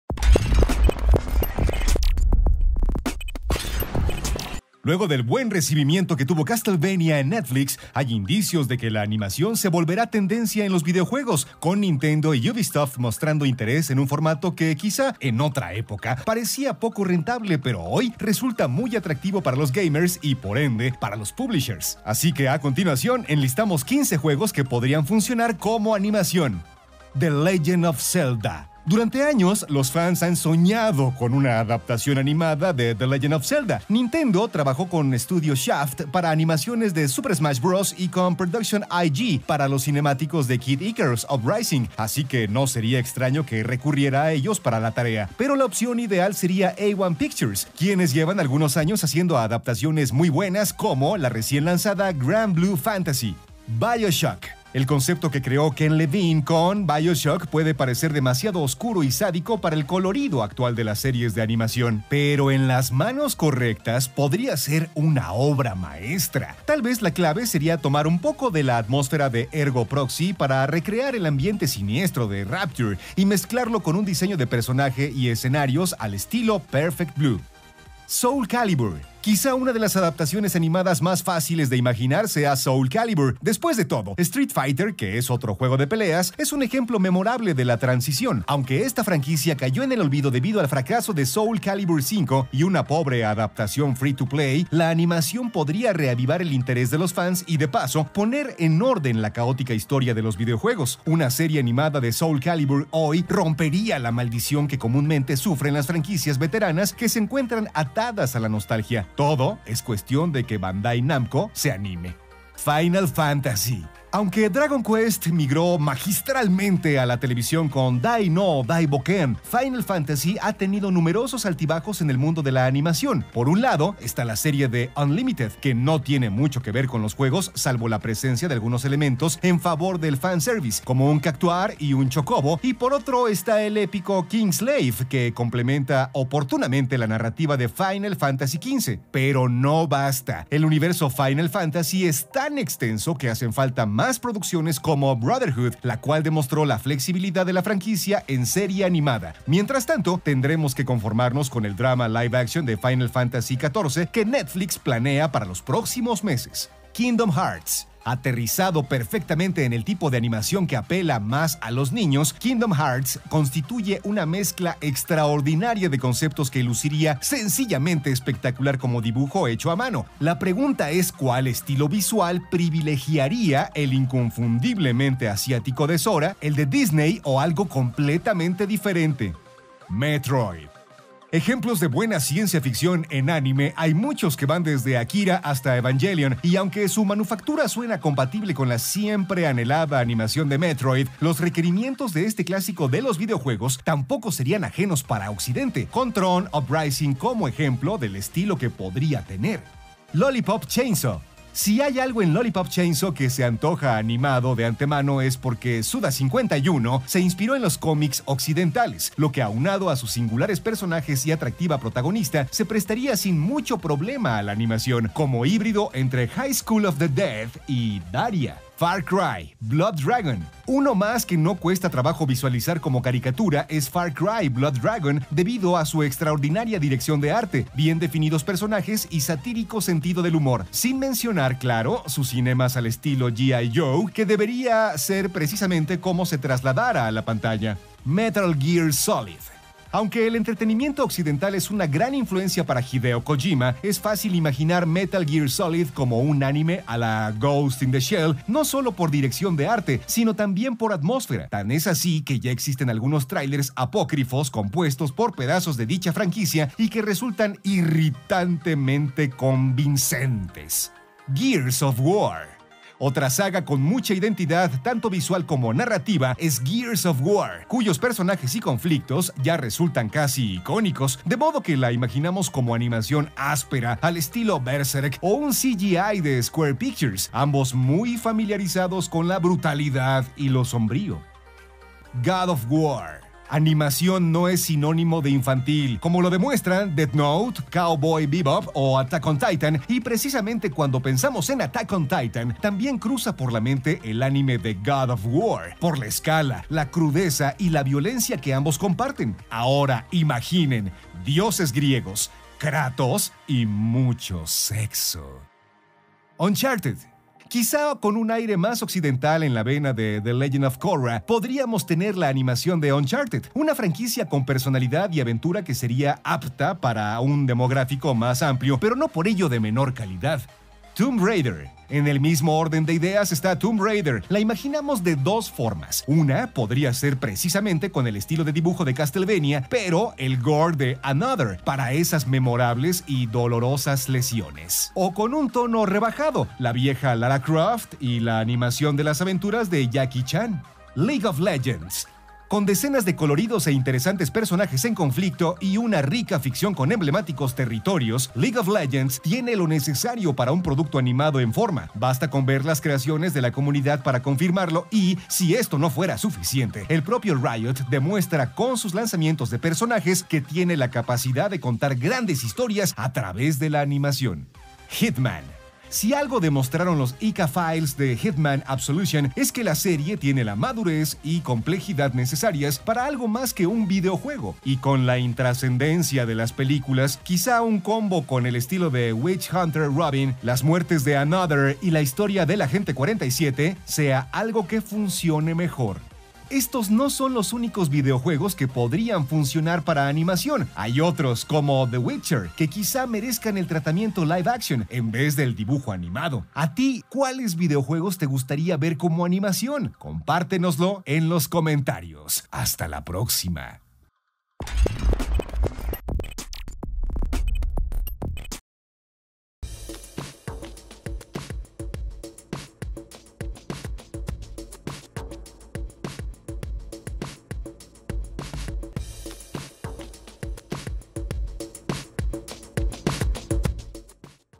15 juegos que merecen una animación
Tras el éxito de Castlevania, nos ponemos a imaginar otros juegos que piden a gritos una versión animada, ya sea japonesa u occidental.

No dejes de darle like a nuestra página oficial en Facebook